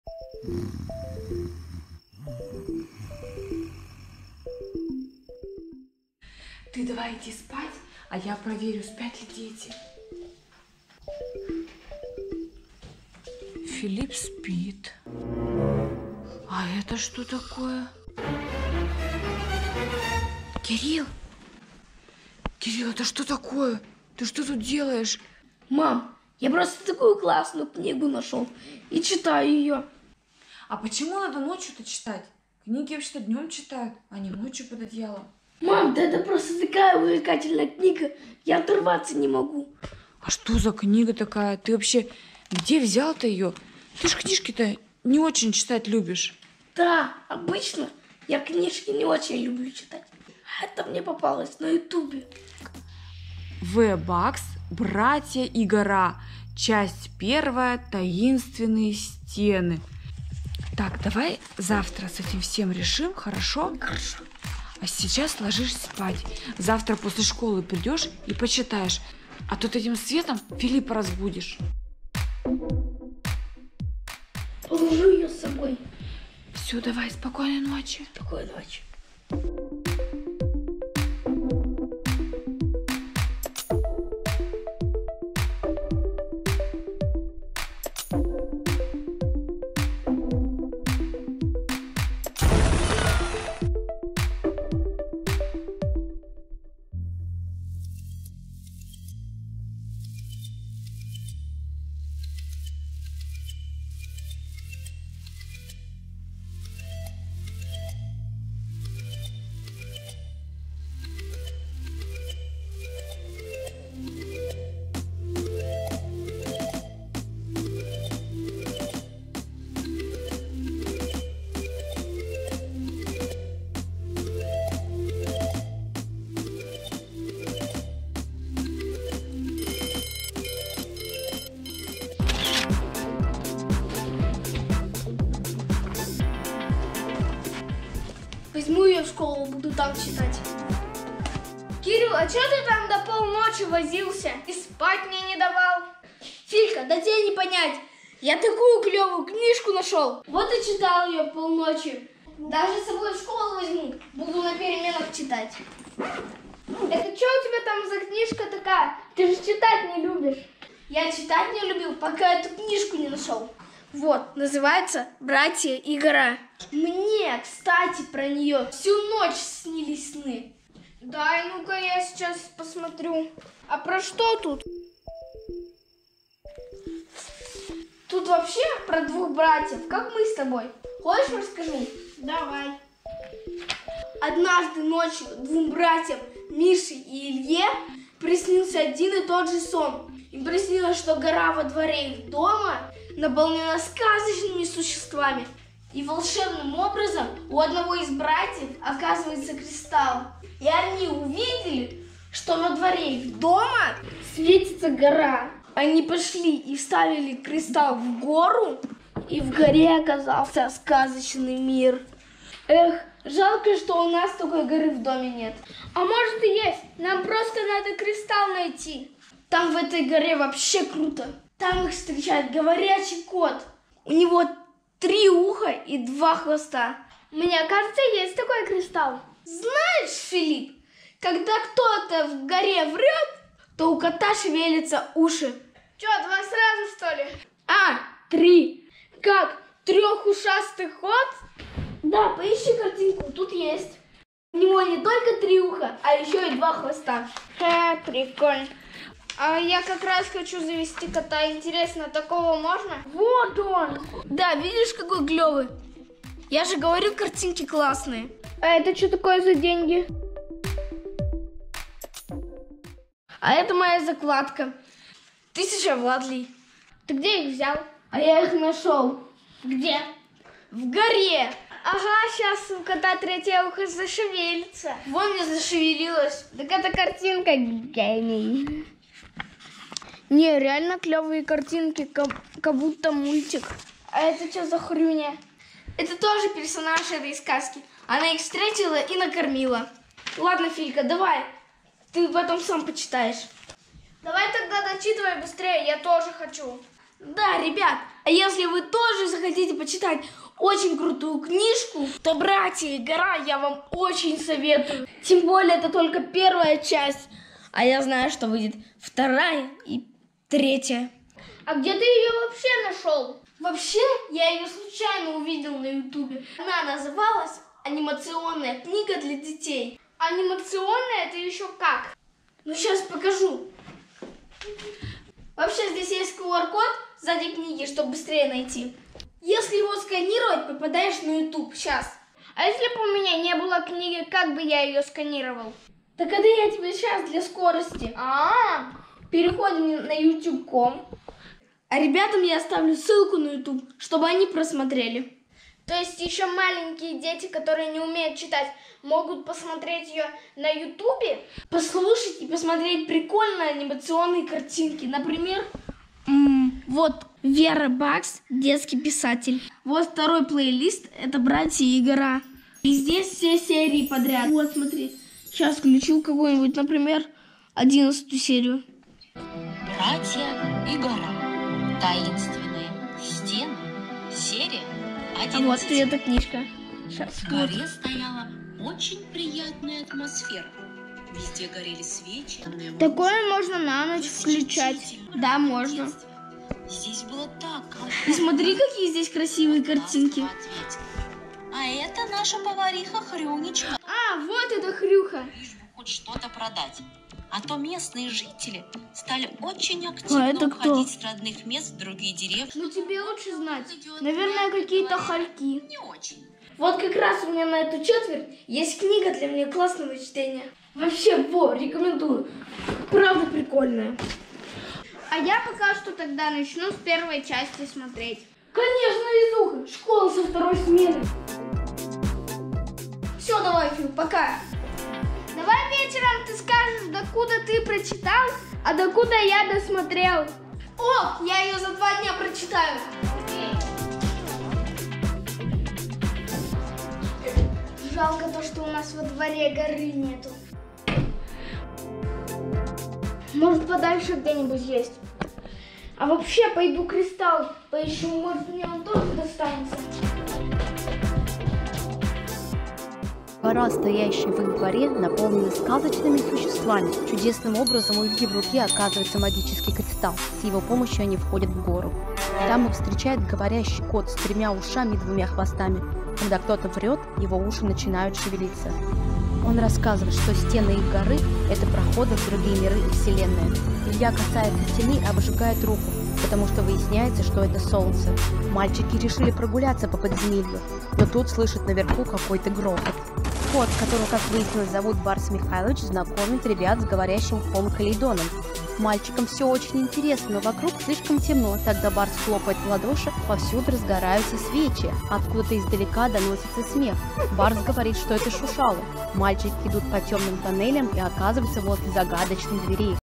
ПОЧЕМУ Кириллу не спится ночью? Братья и Гора / В. Бакс // KiFill
Краткий сюжет истории:
Однажды ночью двум братьям, Мише и Илье, приснился один и тот же сон. Им приснилось, что гора во дворе их дома наполнена сказочными существами. Чудесным образом в руке у Ильи оказывается волшебный кристалл. Мальчики смотрят в окно и видят, что гора светится. Они идут к ней, вставляют кристалл в проем и оказываются в новом, сказочном мире. Их встречает говорящий кот, управляющий горы. Он рассказывает, что эта гора – проход в другие миры и измерения. В данный момент жители столкнулись с большими неприятностями, и им очень нужна помощь братьев. Также мальчики знакомятся с поварихой Хрюней, которая накормила их вкуснейшим завтраком, и многими другими существами. В первой части они успевают совершить героический поступок и спасти девочку Ксюшу, знакомятся с умным мальчиком Ваней и девочкой Аней, которые тоже невероятными путями попадают в гору. И все эти события – только начало большого приключения!